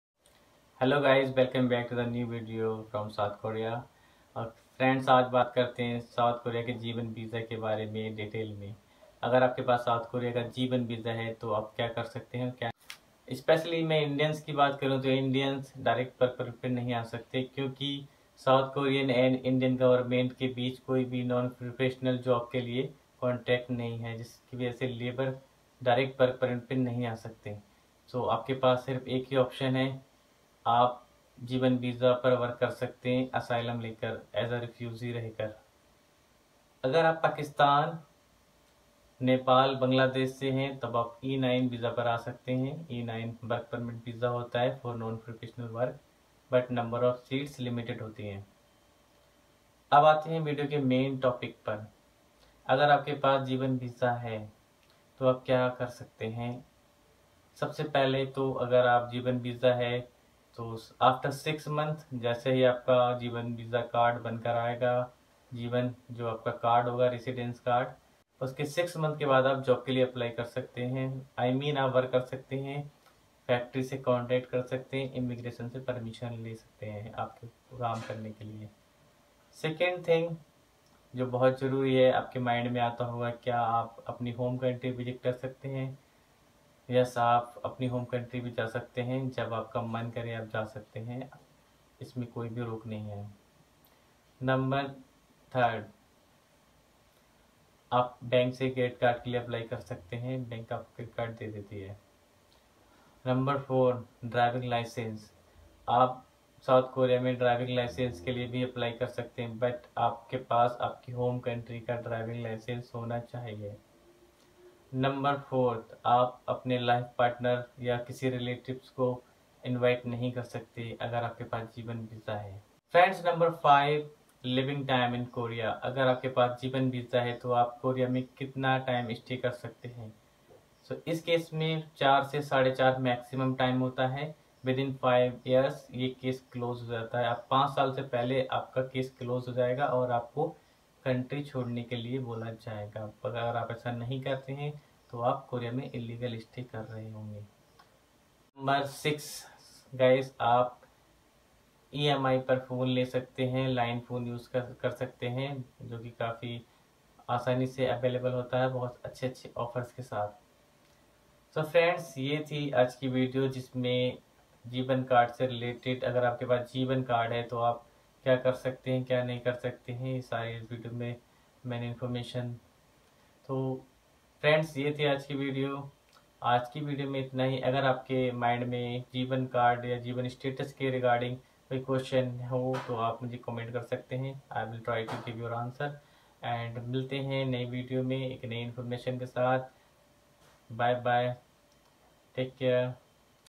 Important information about South Korea 🇰🇷 G-1 visa status, Pros&Cons...
In this video I tried to explain information about G-1 visa in South Korea 🇰🇷